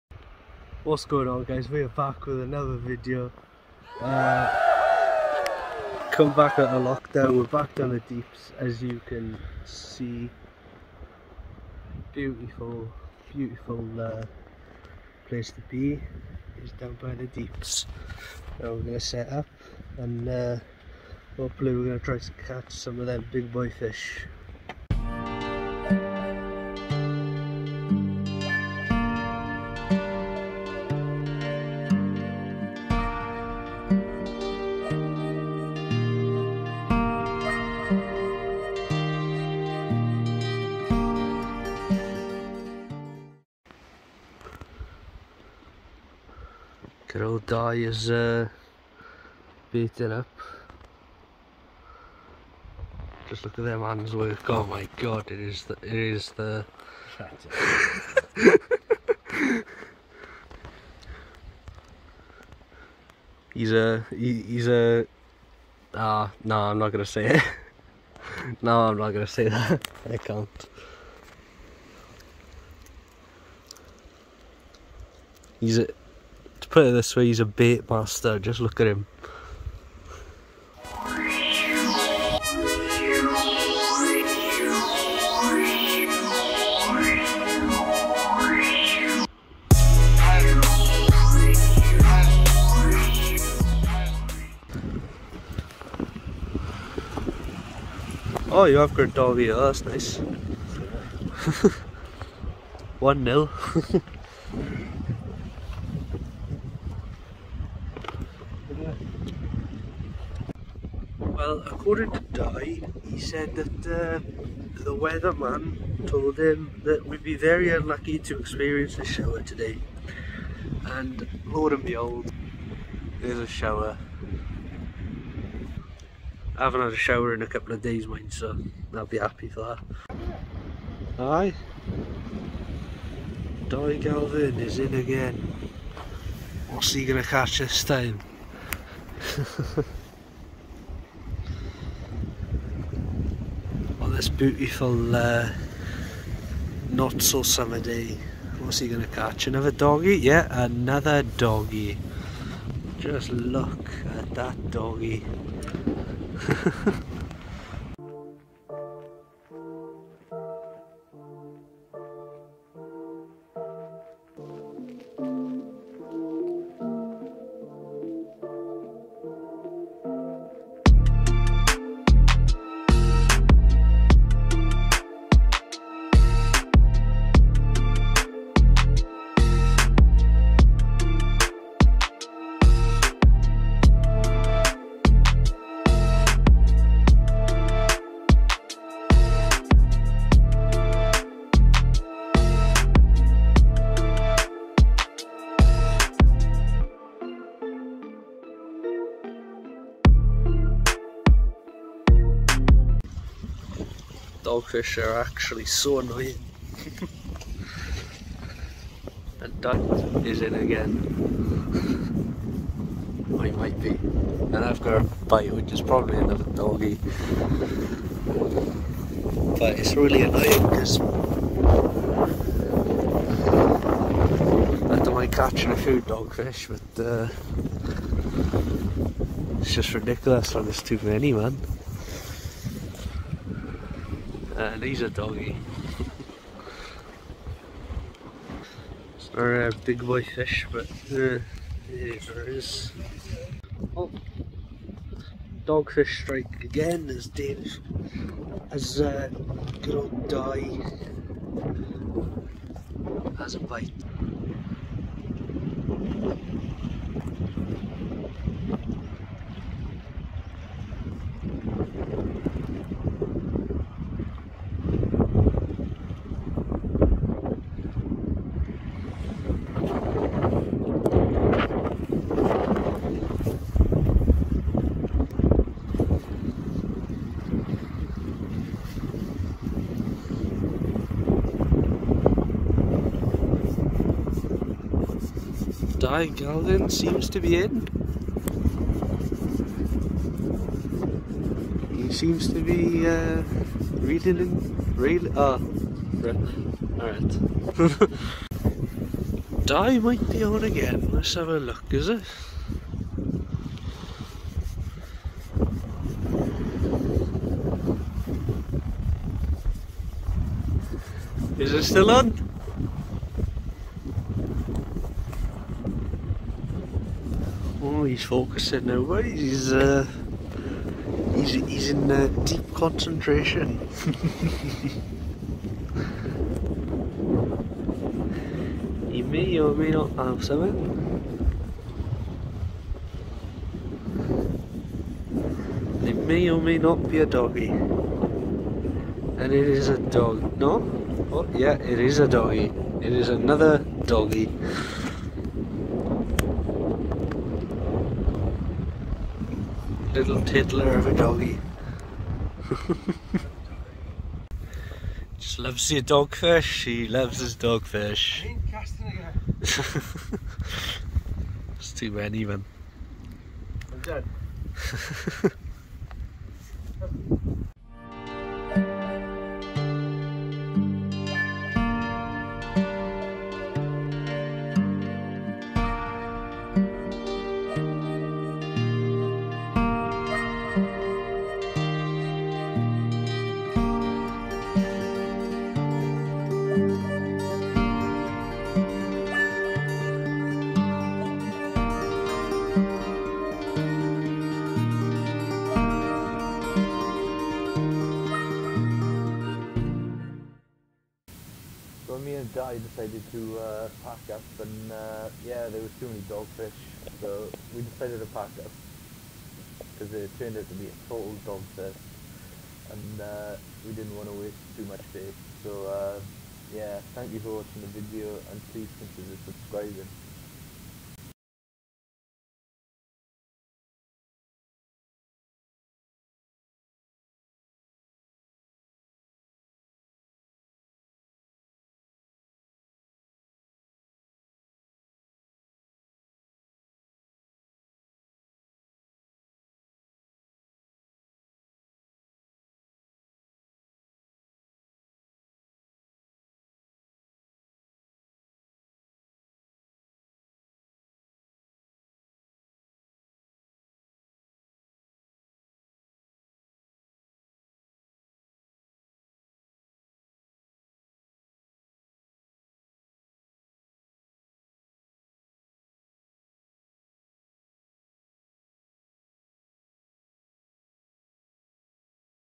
Doggies down the deeps
decided to hit Ogmore deeps last Saturday, was a total dogfish fest